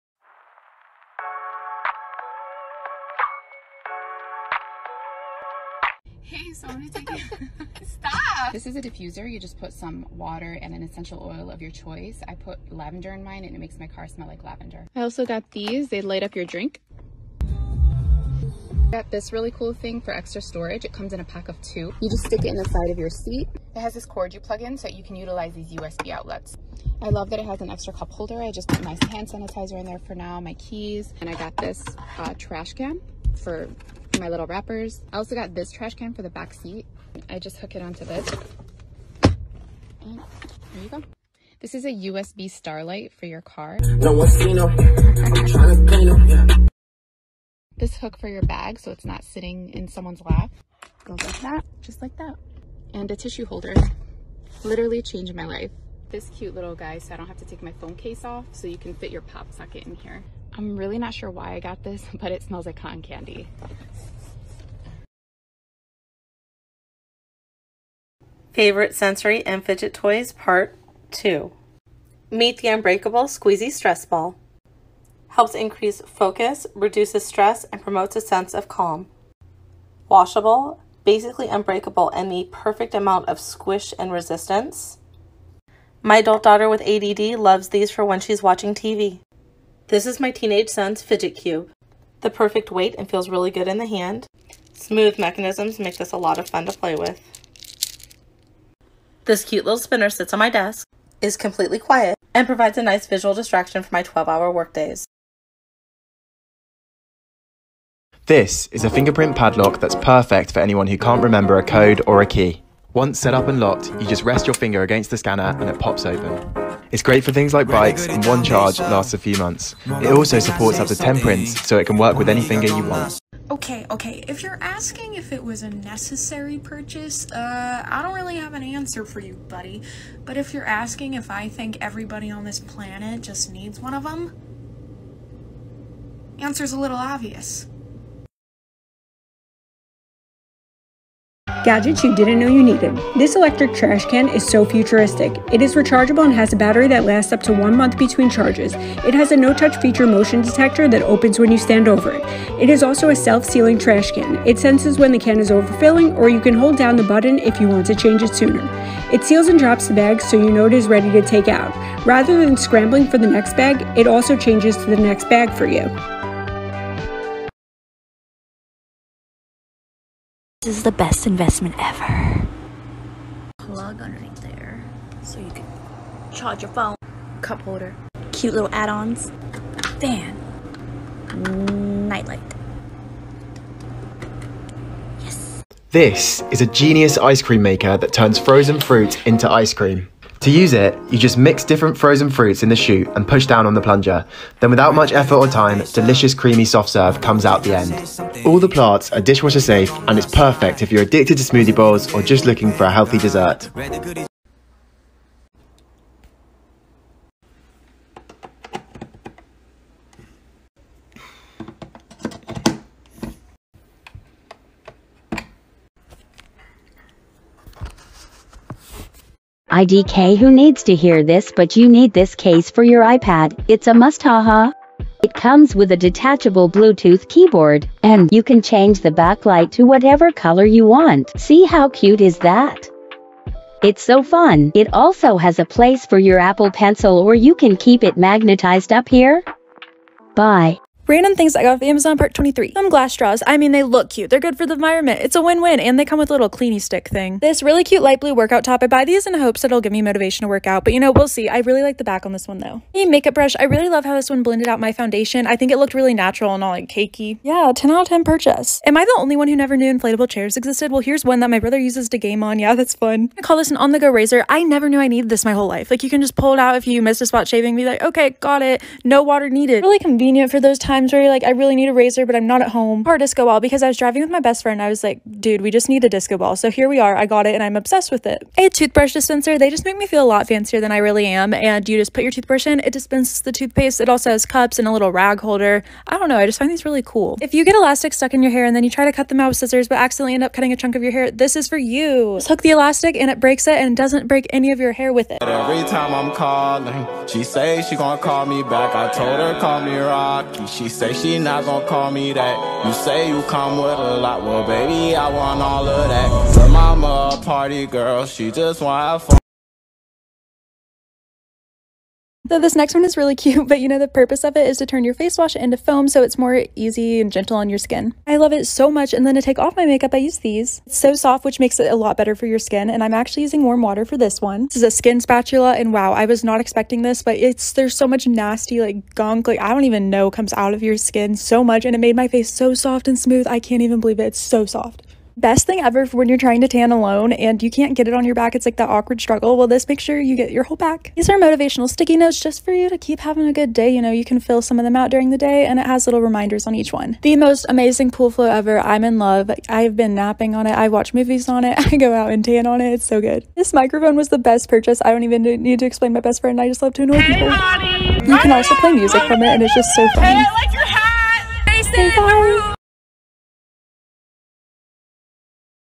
MUST HAVE AMAZON PRODUCTS! WITH LINKS
Links for each one below

Car defuser
light up cup holder
Car extra space holder USB
Mini bin for car
Car bin waterproof
USB star light
Car hook
Car tissue holder
Pop socket car holder
Cotton candy air freshener

Stress balls
Fidget cubes
Fidget spinning ball

Finger print padlock

Pop corn maker

electric self-bagging bin

Laptop accessory table

Ice cream maker

Apple charging dock

Ipad keyboard case

Glass straws
Light blue workout top
Foundation brush
Inflatable chair
On the go razor
Car disco ball
Toothbrush dispenser
Elastic hair band cutter

Hidden pocket scrunchie

Makeup foamer
Microfibre face cloths
Skin spatula skin clean
Pool float canopy